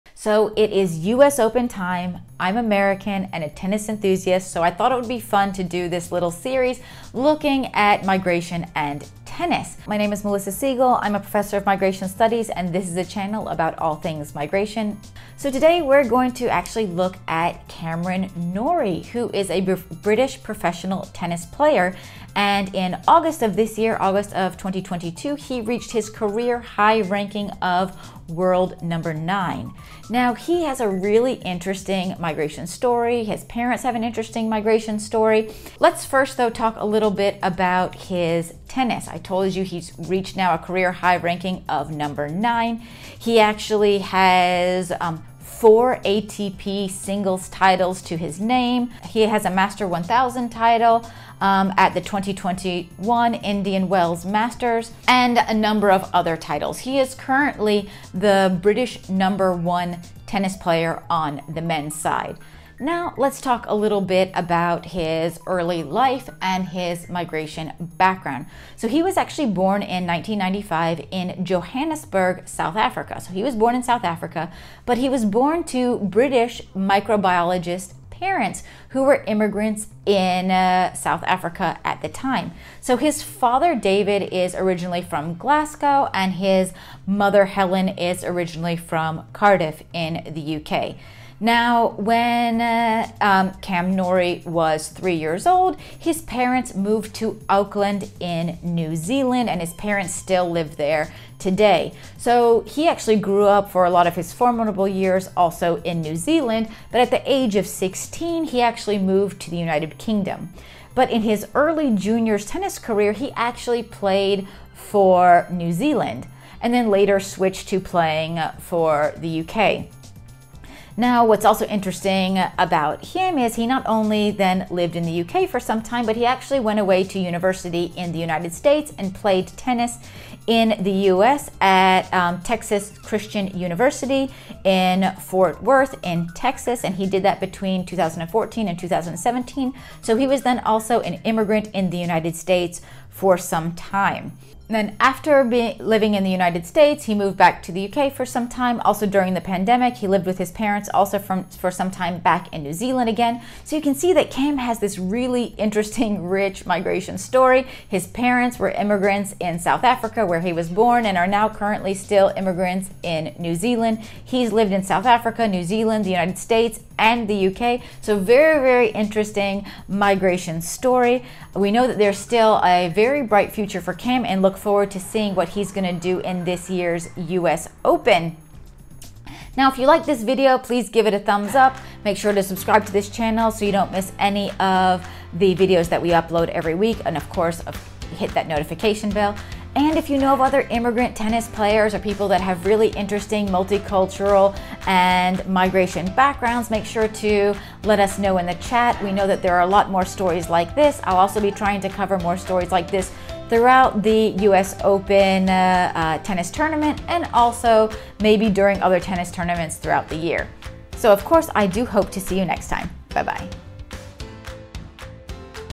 Cameron Norrie: Migration and Tennis
In this video we look at the migration story of the British profession tennis player Cameron (Cam) Norrie. This is part of a series looking at immigrant professional tennis players and the links between migration and tennis.

Links:
Cam Norrie Profile:

Chapters
0:00 Introduction
0:35 Claim to fame
1:02 Migration Background

Stock video and photos courtesy of the creators at pexels.com

Images used from Wikimedia Commons:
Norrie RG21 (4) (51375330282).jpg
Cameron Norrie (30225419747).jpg

Video editing by Kevin O’Dell, UNU-MERIT Researcher